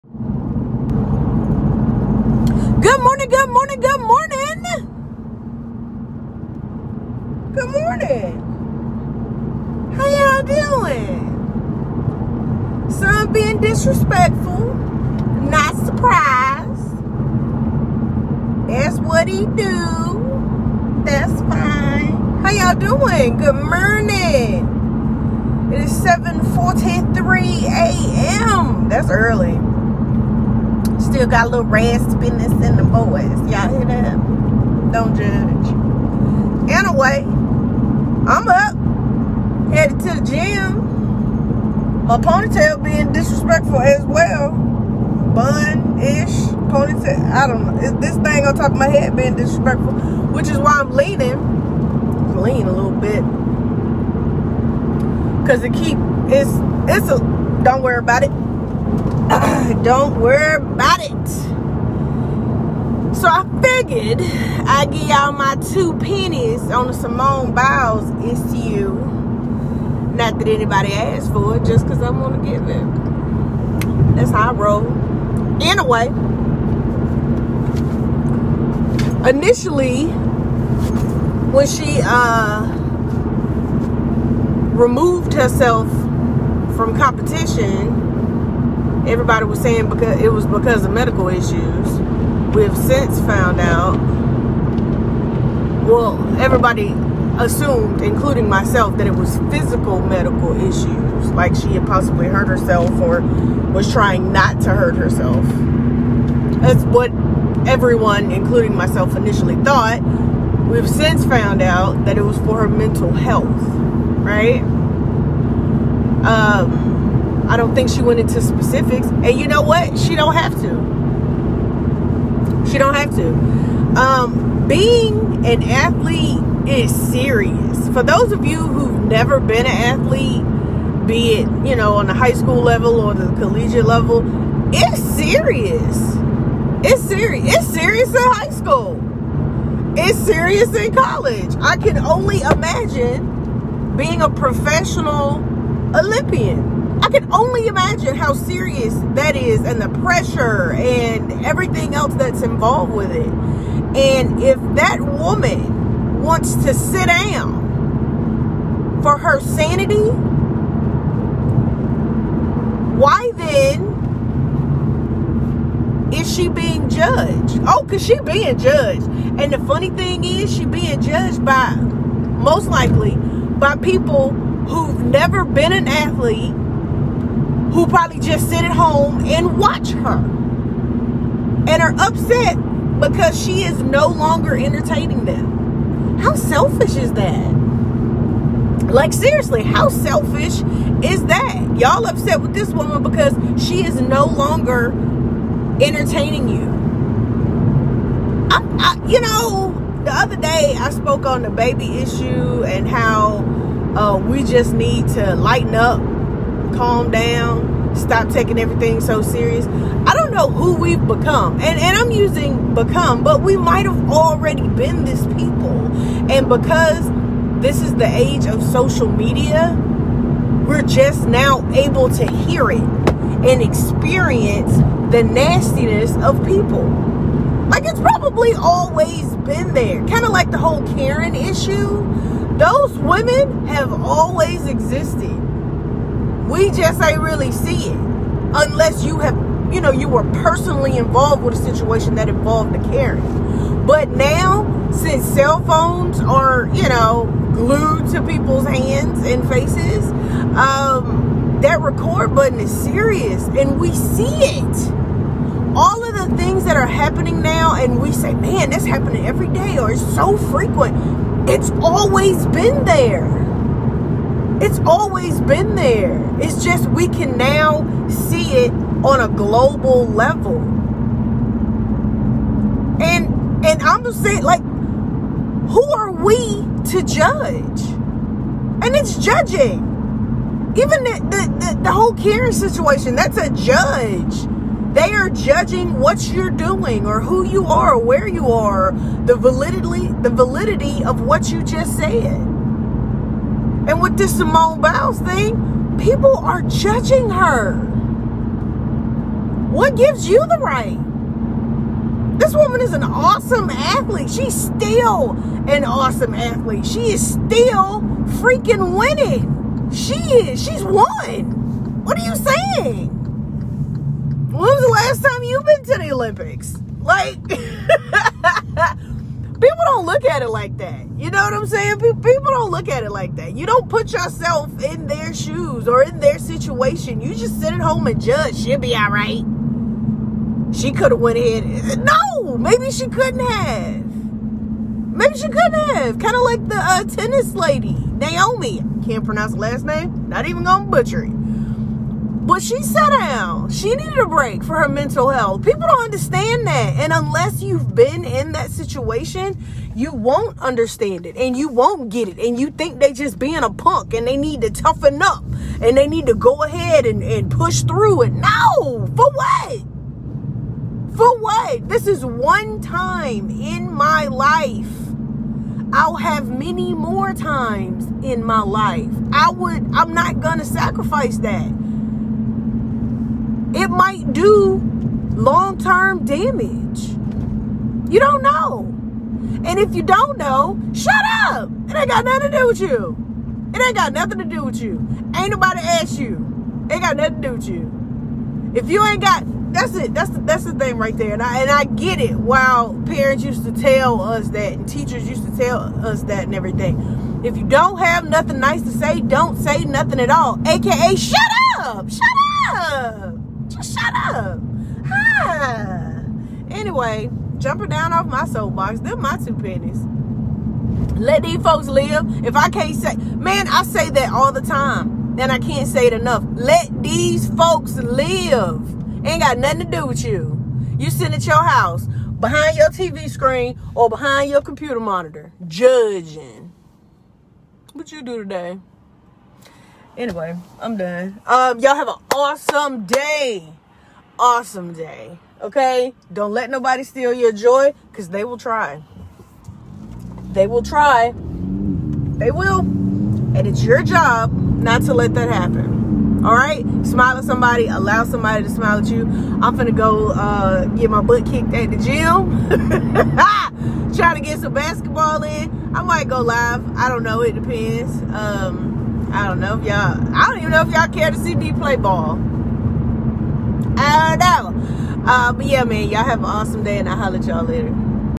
Simone Biles - Thursday Morning Live (7/29/21)
My 2 pennies on Simone Biles sitting down..